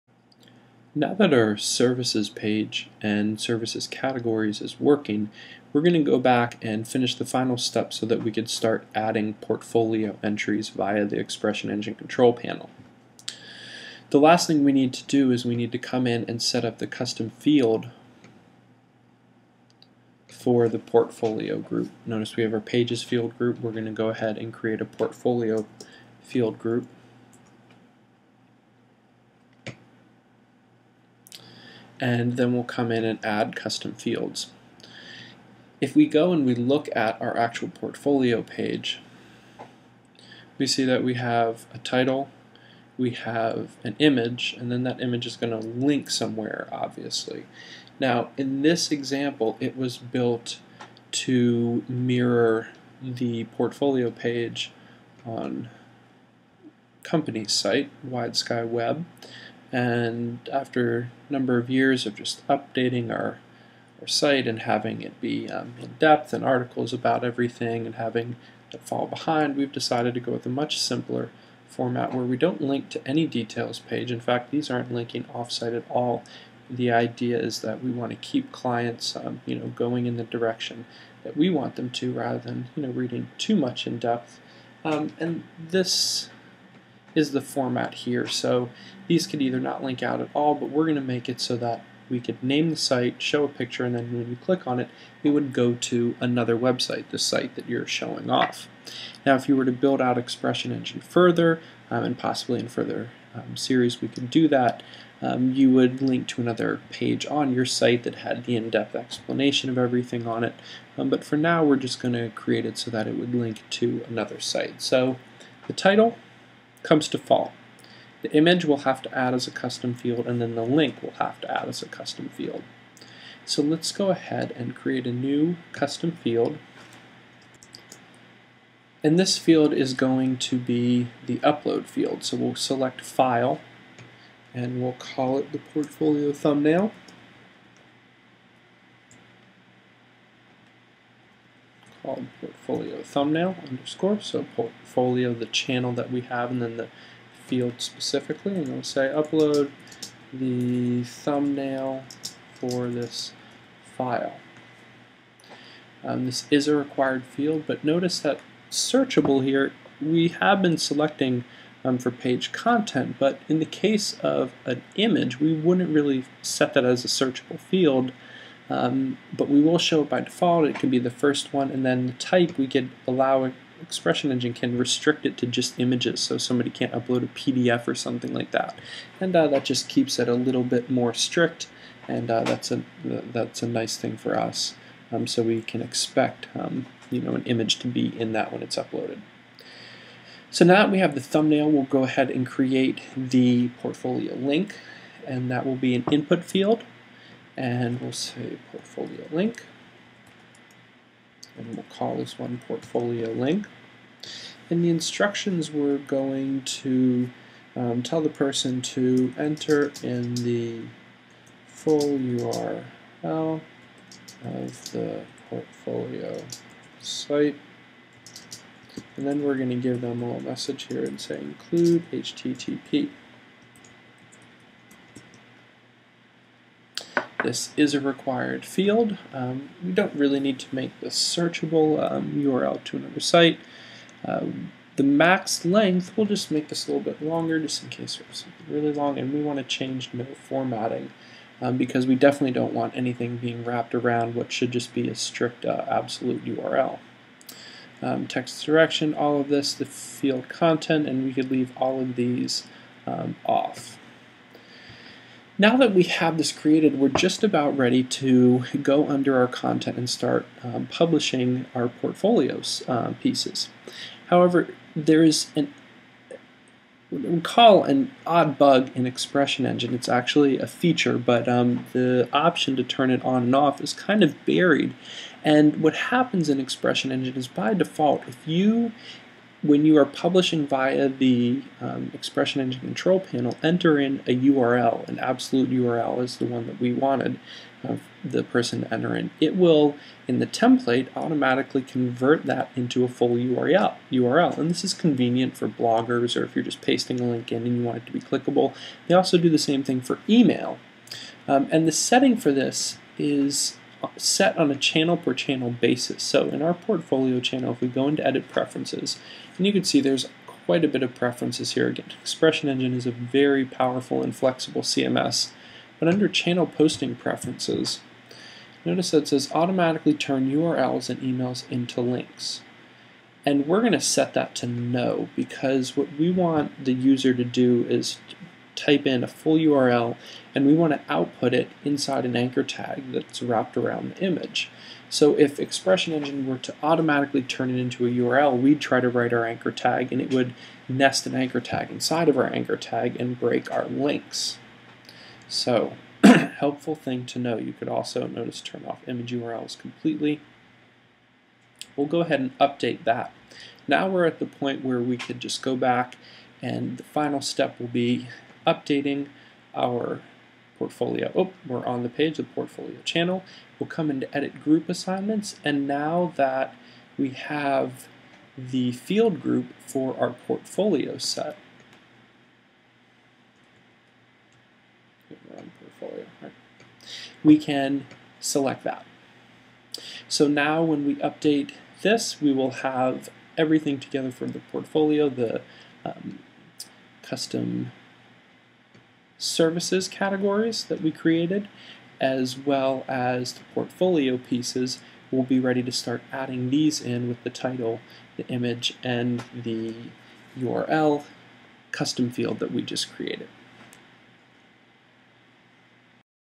Creating the Custom ExpressionEngine Fields for the Portfolio Page (22/25) DaBrook EE Tutorial
In this video we go over how to create the custom fields for the Portfolio section of our site.  We also talk about the important setting of automatically turning URLs into links.